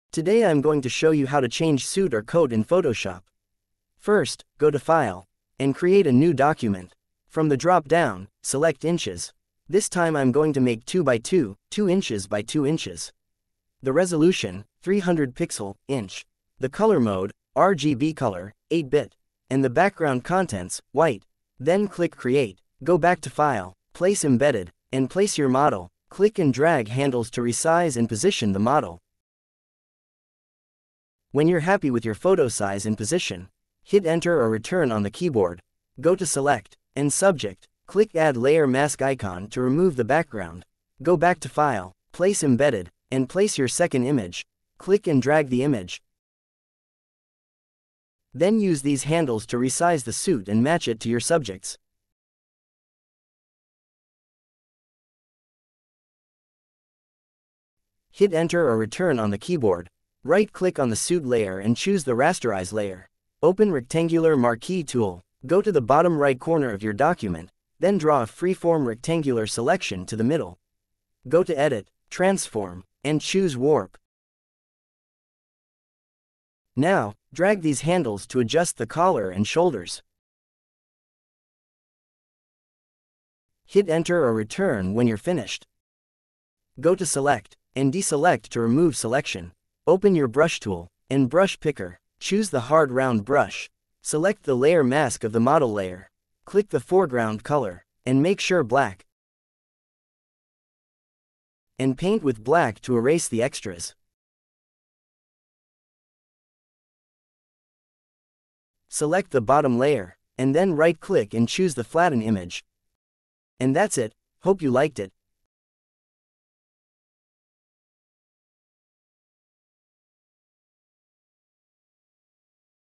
How to create 2x2 with formal attire - Photoshop Tutorial
In this 2x2 picture photoshop tutorial, learn how to create 2x2 picture in photoshop. I will also show you step by step how to create 2x2 with formal attire in photoshop. I hope this lesson helps.

✅Easy way to remove the white background on your drawing and make it transparent - Photoshop Tutorial